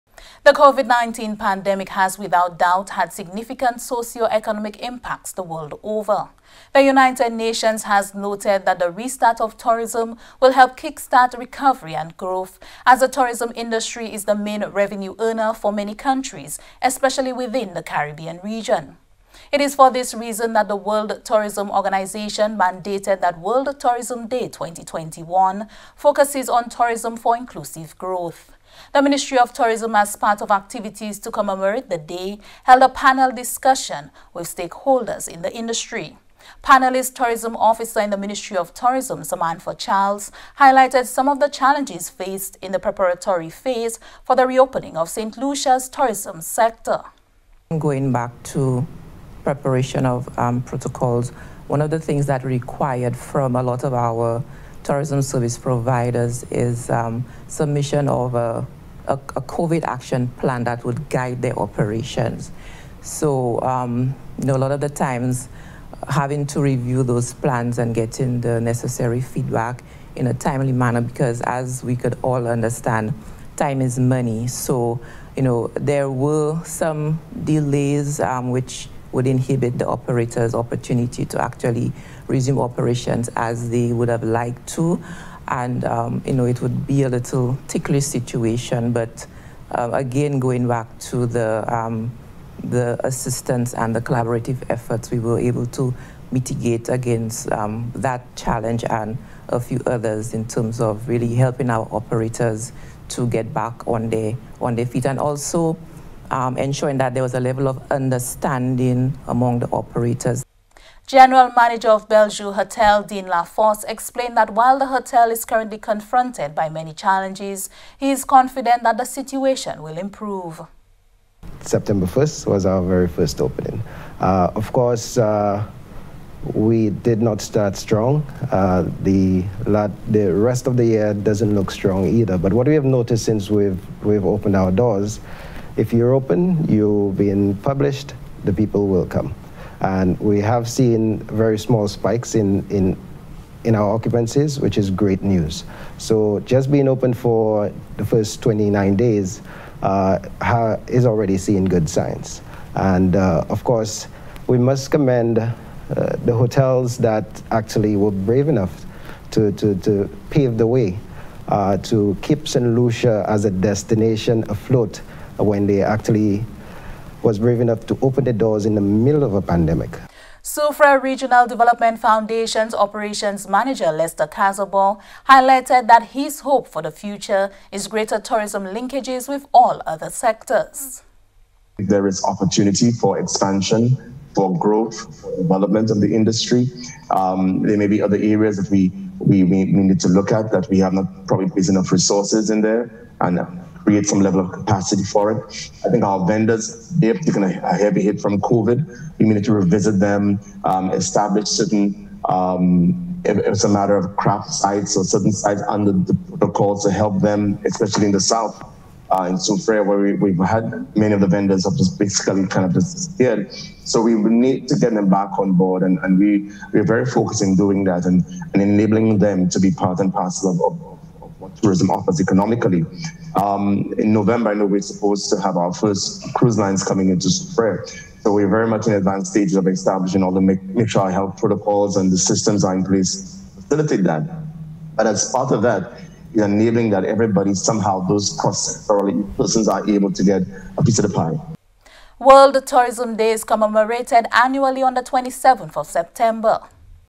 Reopening of Saint Lucia's tourism sector
The COVID-19 pandemic has without doubt had significant socio-economic impacts the world over. The United Nations has noted that the restart of tourism will help kick start recovery and growth as the Tourism industry is the man revenue earner for many countries especially within the Caribbean region. It is for this reason that the World Tourism Organization mandated that World Tourism Day 2021 focuses on “Tourism for Inclusive Growth.”
The Ministry of Tourism as part of activities to commemorate the day held a panel discussion with stakeholders in the industry. Tourism Officer in the Ministry of Tourism, Samantha Charles highlighted some of the challenges faced in the preparatory phase for the reopening of Saint Lucia’s tourism sector.